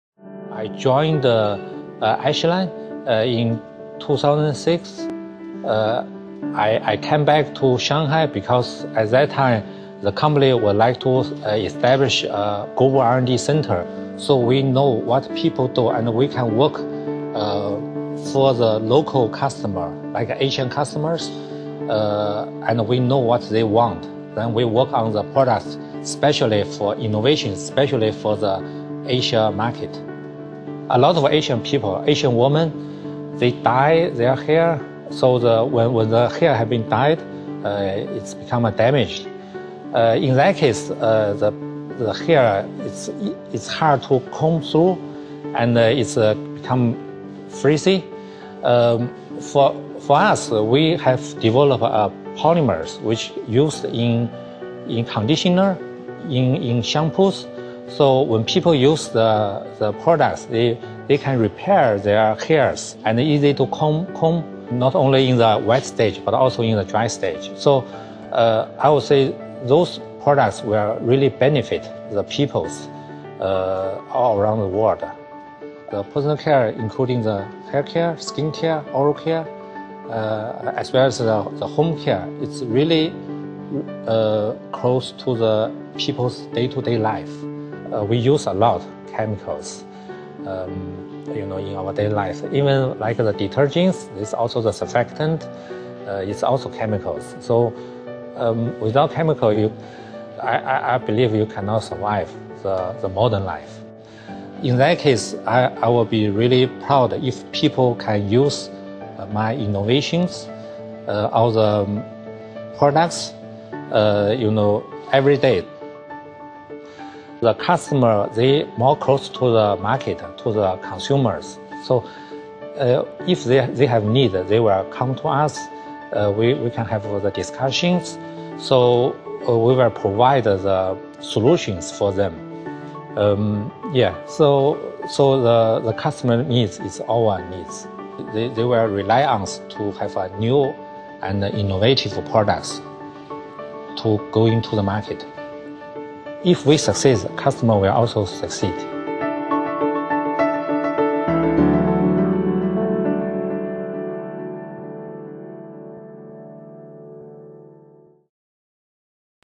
Qu Xin, research and development lab manager
A childhood interest in chemistry can lead to a career of developing breakthrough products. Dr. Qu Xin leads our lab focusing on personal care for customers in the Asia Pacific region.